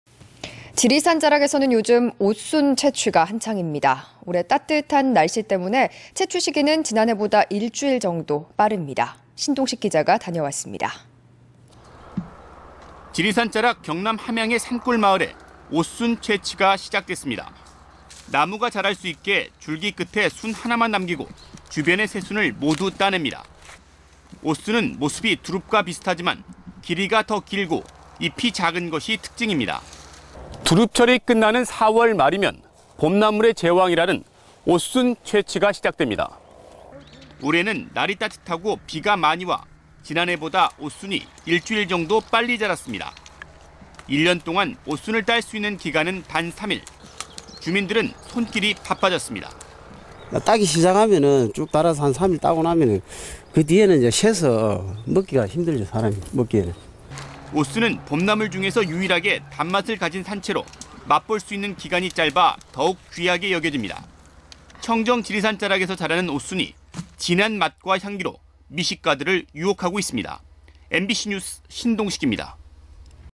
MBC경남 뉴스데스크 2016 04 29 지리산 옻순 채취 한창
지리산 자락에서는 요즘
옻순 채취가 한창입니다.

올해는 따뜻한 날씨에 채취 시기가
지난해보다 일주일 정도 빠릅니다.

신동식 기자

지리산 자락 경남 함양의 산골마을에
옻순 채취가 시작됐습니다.

나무가 자랄 수 있게
줄기 끝에 순 하나만 남기고
주변에 새순을 모두 따냅니다.

옻순은 모습이 두릅과 비슷하지만
길이가 더 길고 잎이 작은 것이 특징입니다.

두릅철이 끝나는 4월 말부터
봄나물의 제왕으로 불리는 옻순 따기가
시작됩니다.

올해는 날이 따뜻하고 비가 많이와
지난해보다 옻순이 일주일 정도
빨리 자랐습니다.

일년 동안 옻순을 딸 수 있는 기간은 단 3일,
주민들은 손길이 바빠졌습니다.

장철안
"옻순은 일정크기 이상으로 자라면 먹을 수가 없습니다."

옻순은 봄나물 중에서 유일하게
단맛을 가진 산채로 맛볼 수 있는 기간이 짧아
더욱 귀하게 여겨집니다.

청정 지리산 자락에서 자라는 옻순이
진한 맛과 향기로 미식가들을 유혹하고
있습니다.

MBC NEWS 신동식